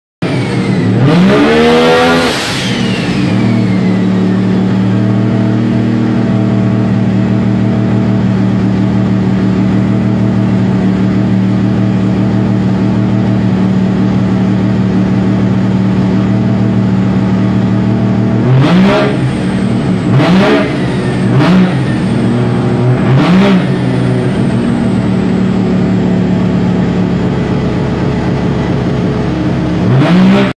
sdr motorsport 20B
20B running E85 on the dyno